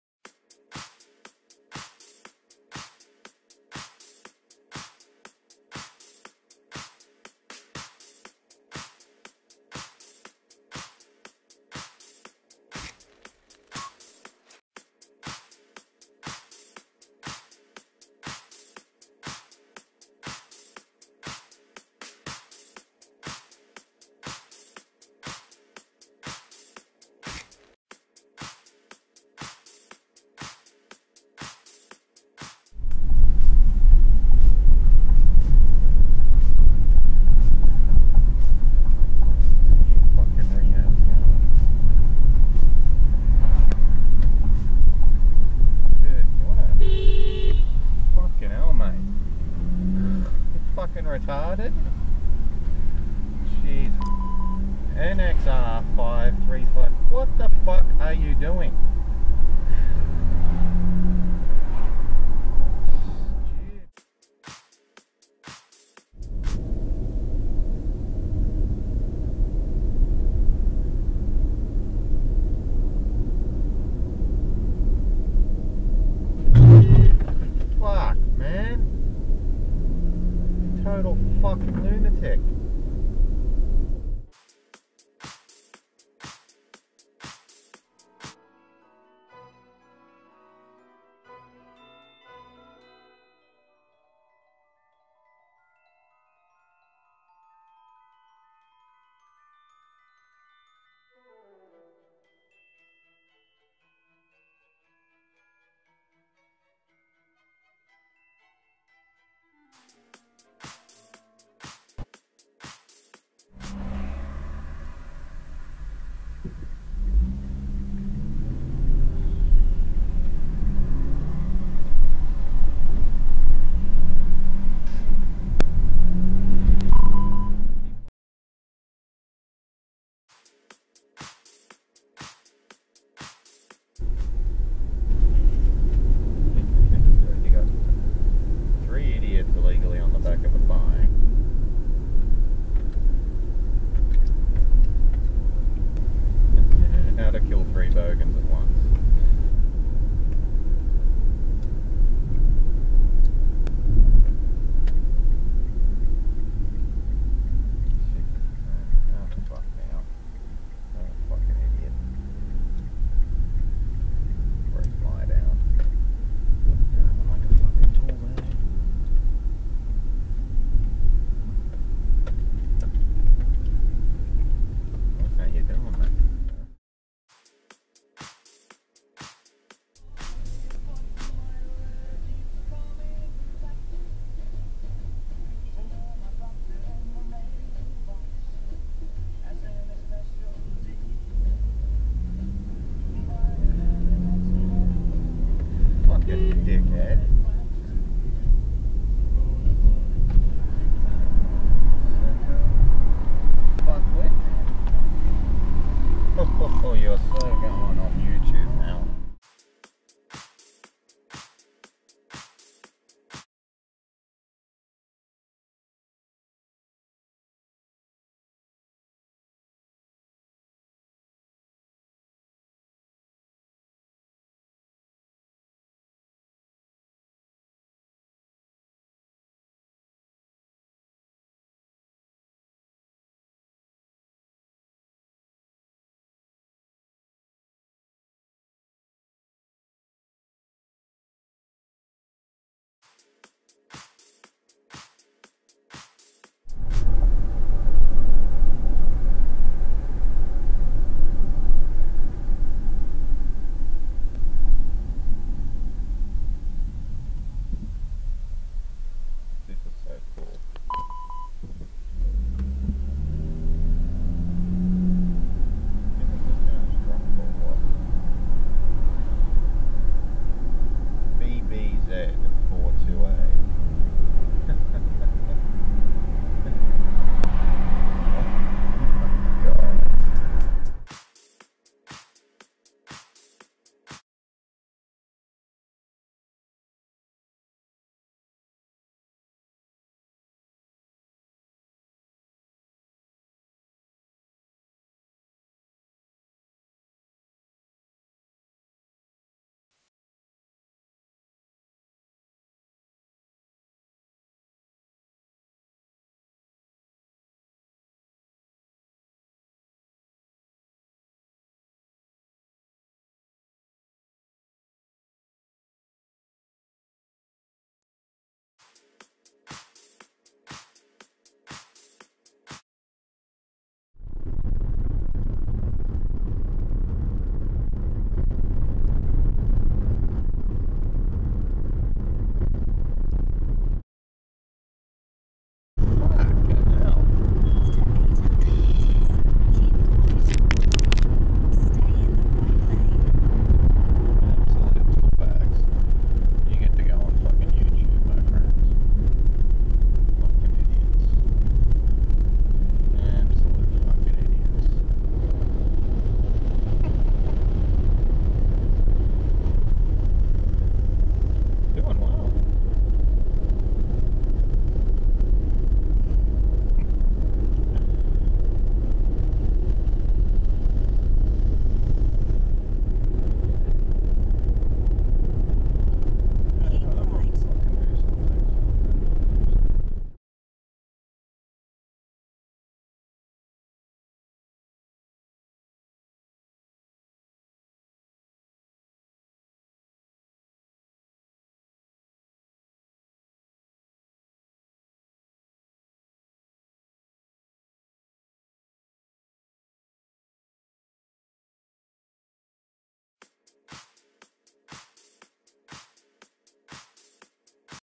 TOP 10 Worst Sydney Driver's Of 2015
Hi everyone This is my third Annual top 10 worst drivers video .

Here are the links to my previous two top 10 videos 2013 & 2014 please check them out .
Also the links below are the full length videos for each number in the video (some are in compilation videos)